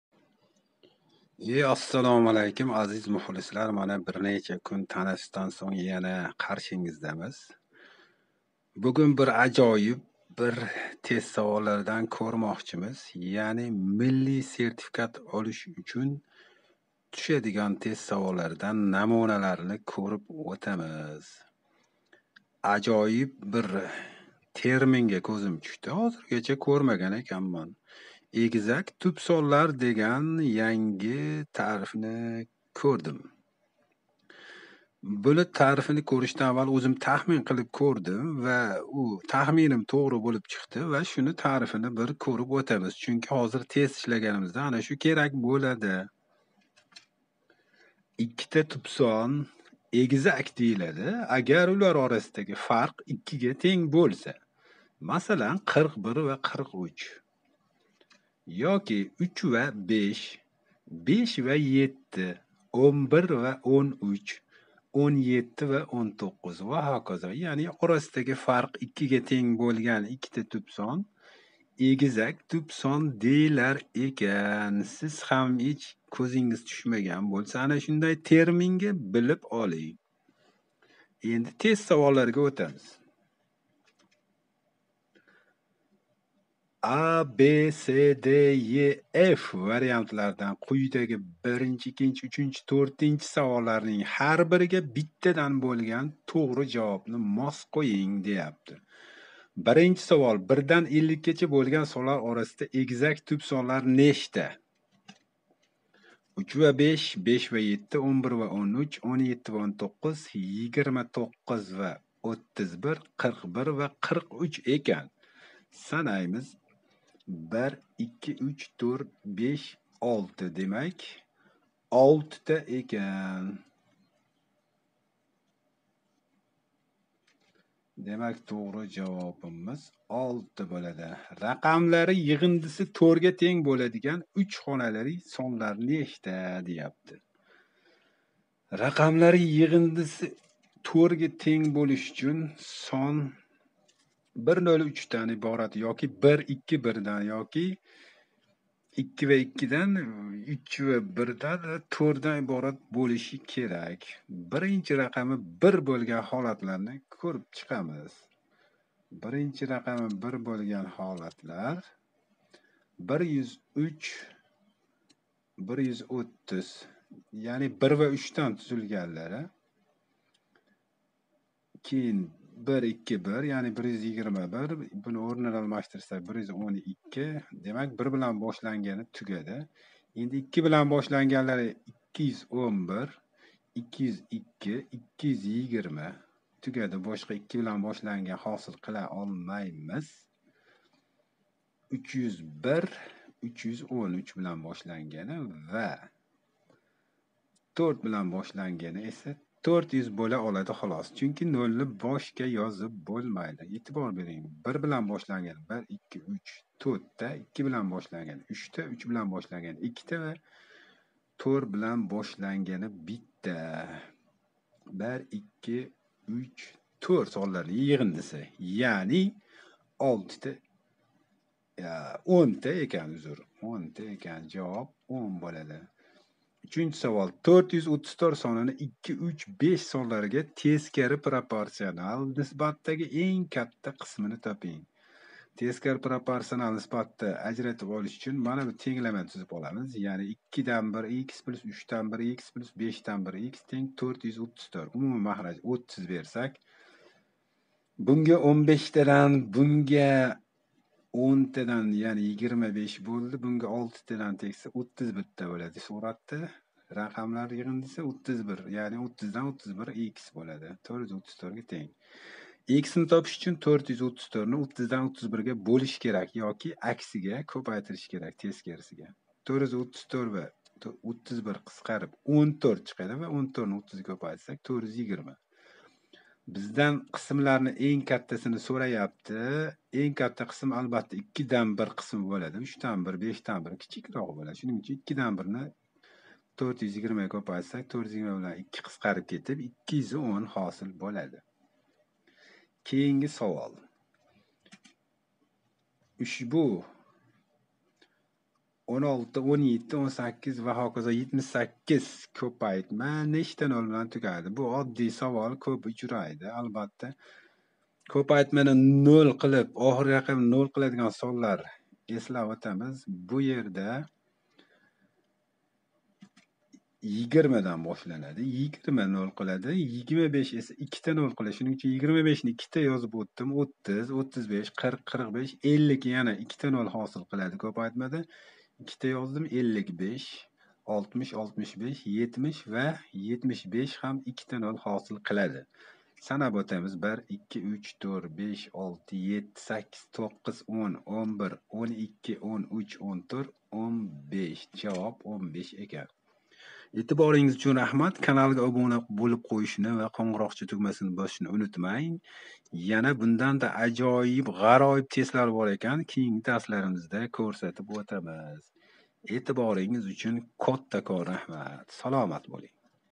Milliy sertifikat savollari | Milliy test. Matematika
Milliy sertifikat olish uchun beriladigan savollardan namunalar. Egizak tub sonlar. Testlar yechimi